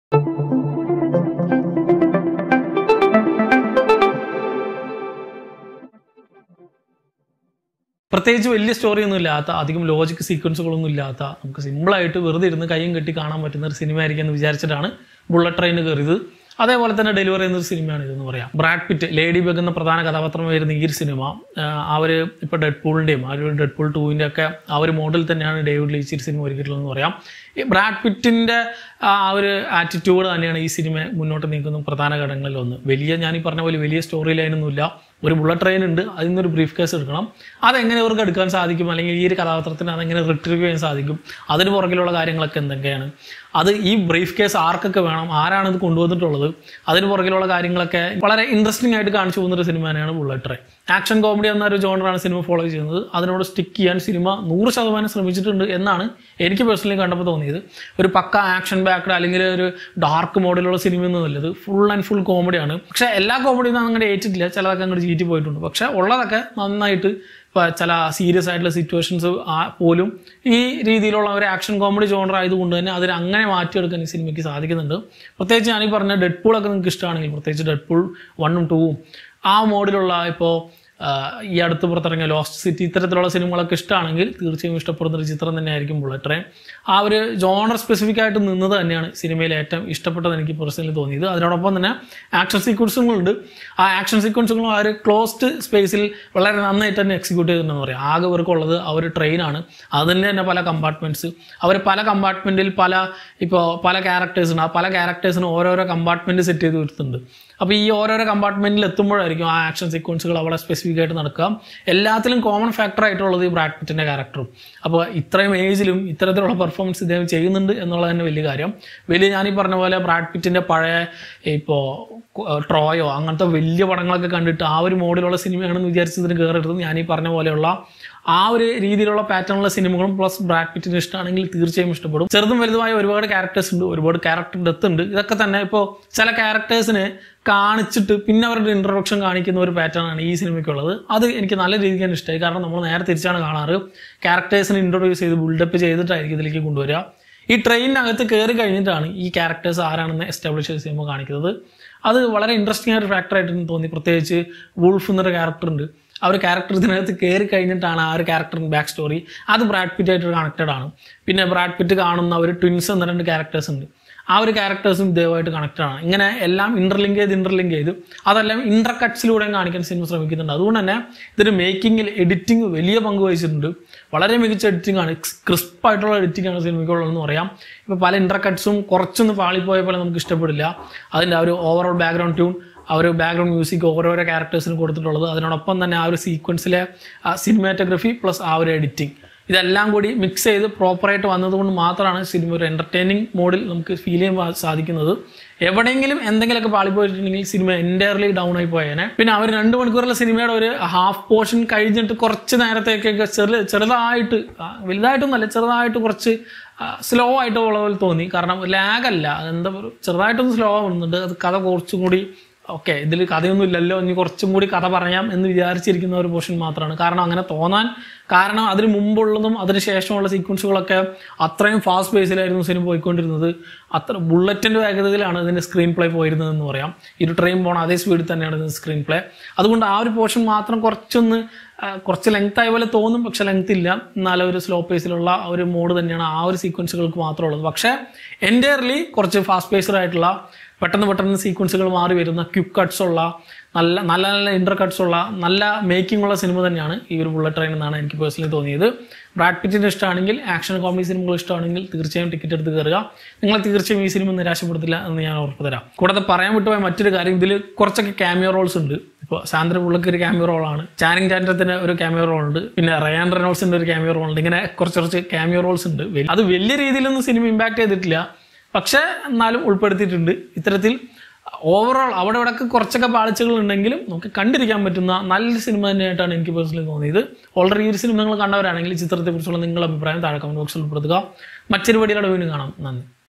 Bullet Train Movie Malayalam Review | Brad Pitt | Reeload Media
Bullet Train is a 2022 American action comedy film directed by David Leitch from a screenplay by Zak Olkewicz, based on the Japanese novel Maria Beetle by Kōtarō Isaka. The film stars an ensemble cast including Brad Pitt, Joey King, Aaron Taylor-Johnson, Brian Tyree Henry, Andrew Koji, Hiroyuki Sanada, Michael Shannon, Benito A Martínez Ocasio, and Sandra Bullock.